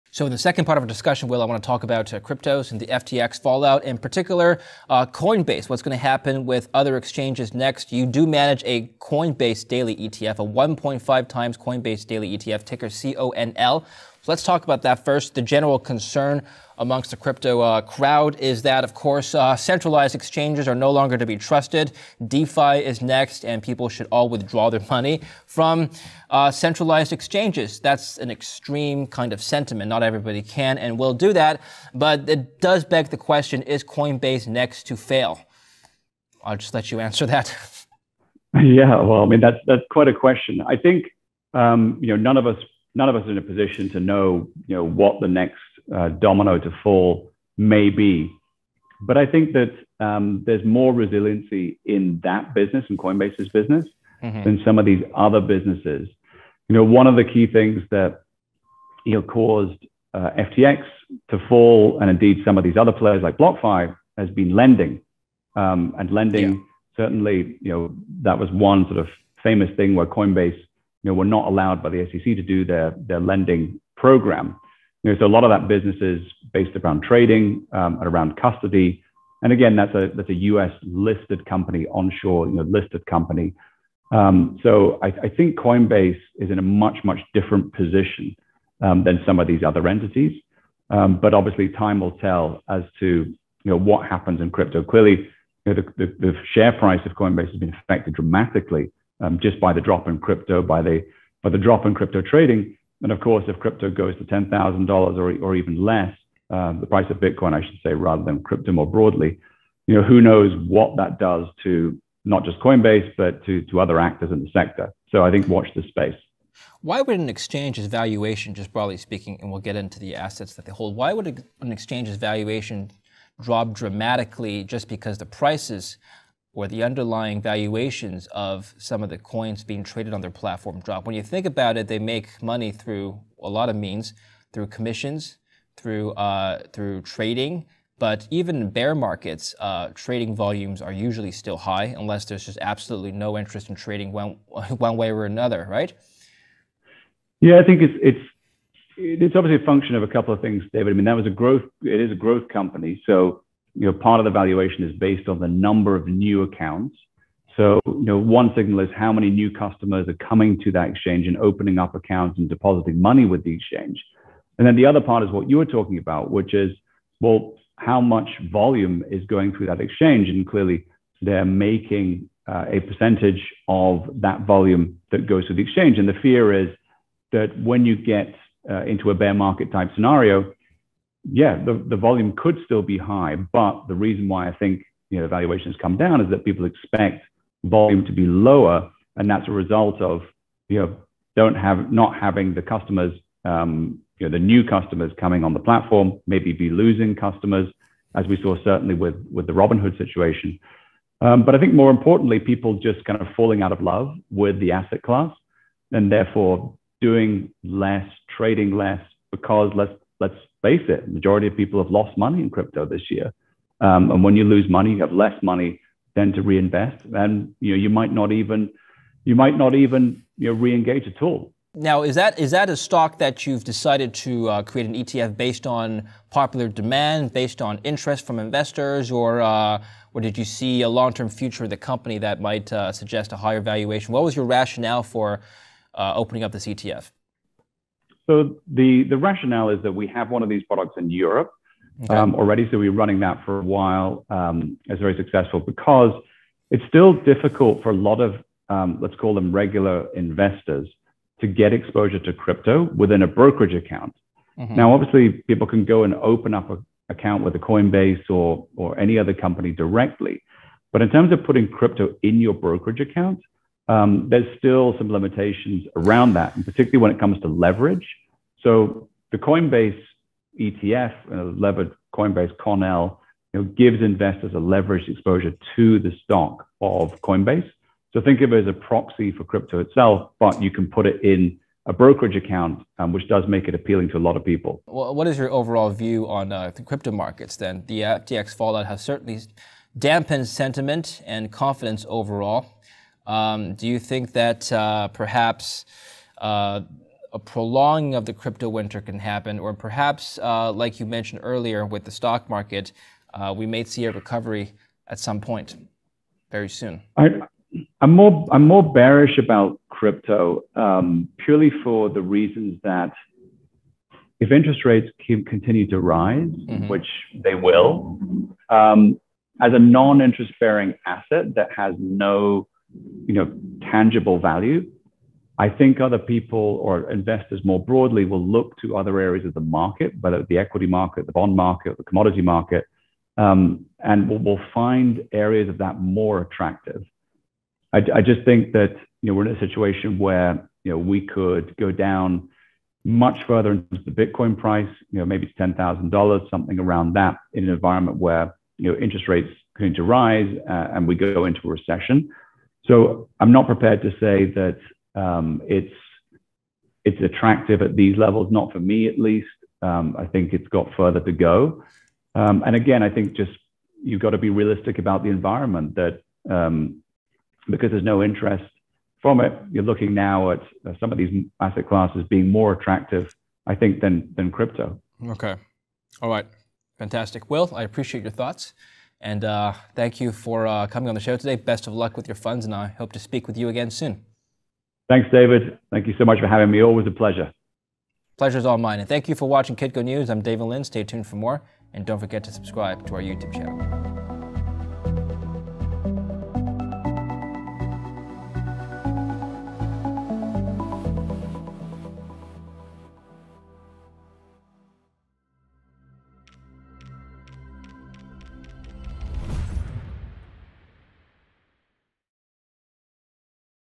Is Coinbase next to collapse after FTX? Crypto Winter has 'further to go' - Will Rhind (Pt. 2/2)
Will Rhind, CEO of GraniteShares discusses Coinbase and the outlook for cryptos with David Lin, Anchor for Kitco News.